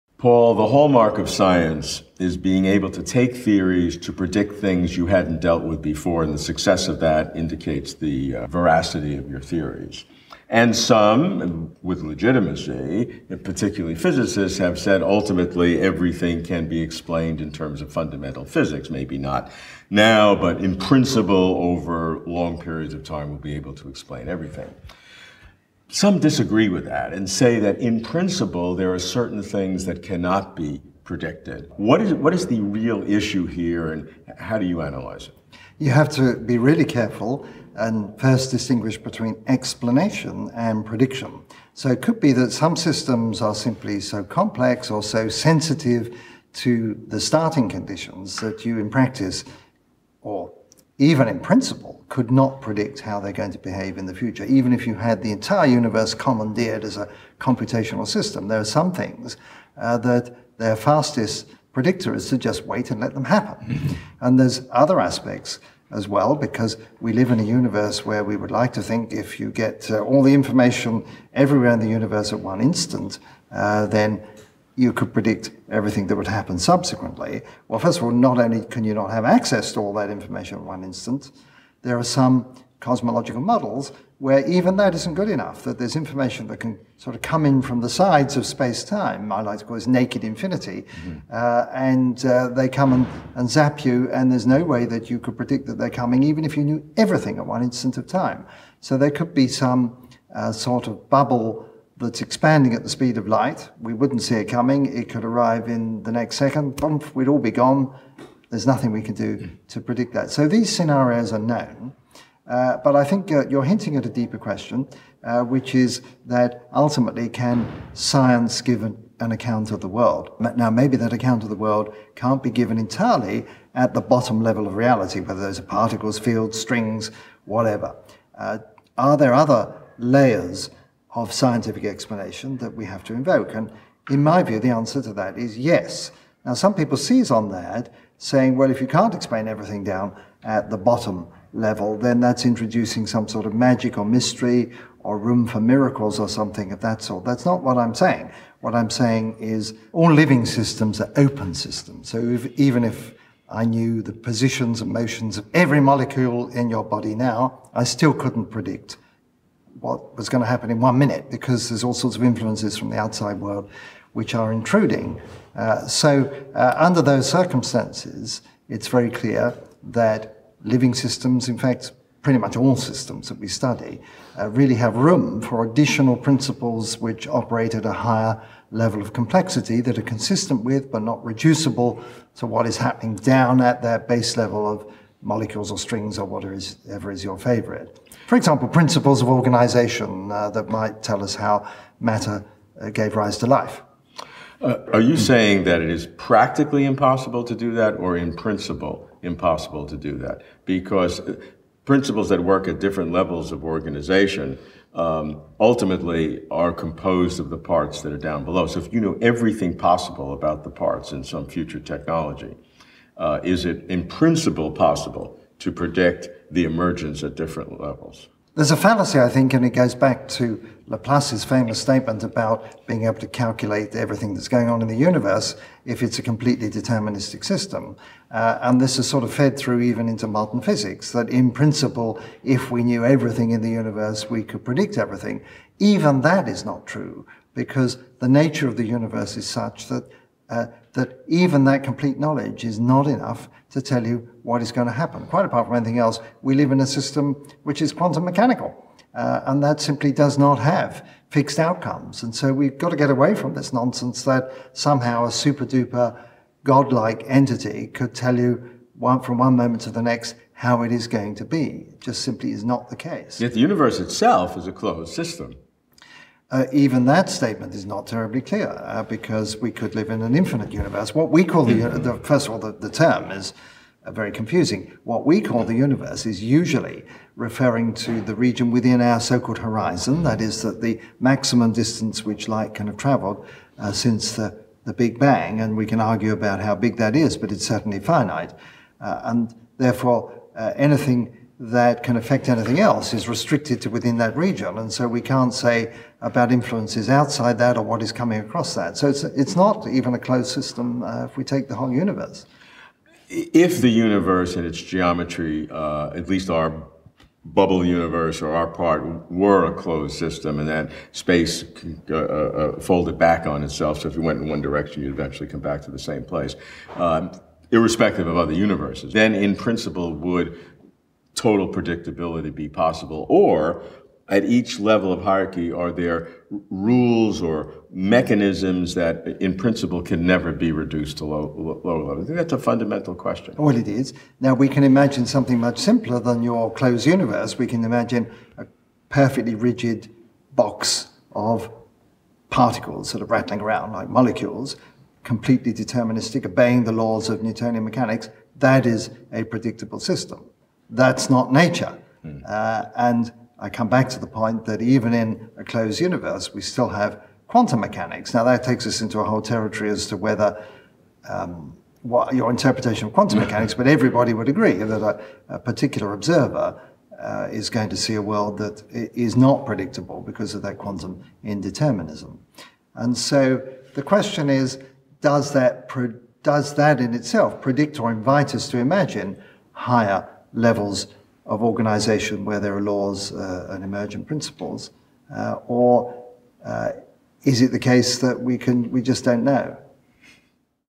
Paul Davies - What Can't Be Predicted in Physics?
Prediction is the fruitful product of good science, but how far can prediction go? Physics is the most mathematical and rigorous of the sciences and so prediction is most successful in physics. But are there limits to predictability in physics? What about quantum indeterminacy? Are there ultimate barriers to prediction in physics?

Paul Davies is a theoretical physicist, cosmologist and astrobiologist.